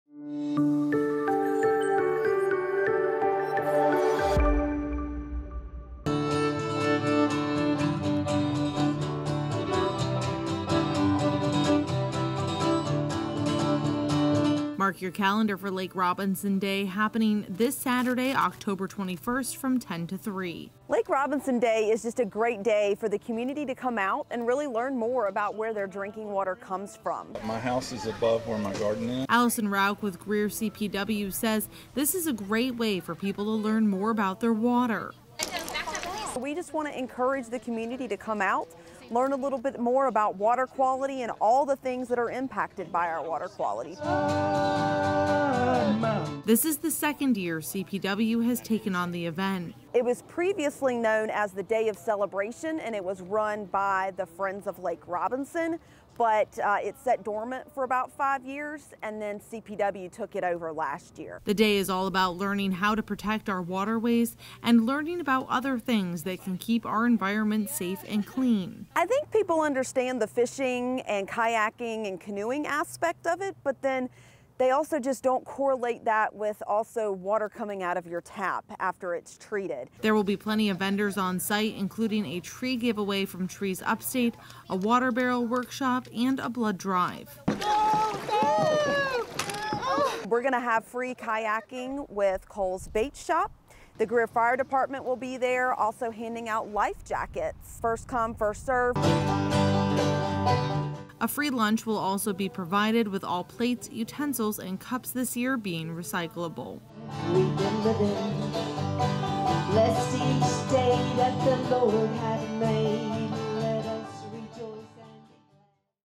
LAKE ROBINSON DAY '23 PREVIEW
Lake Robinson Day is coming up this weekend with Greer CPW! Come join us from 10 AM - 3 PM at Lake Robinson for a day full of fun, and to learn more about our water system in Greer.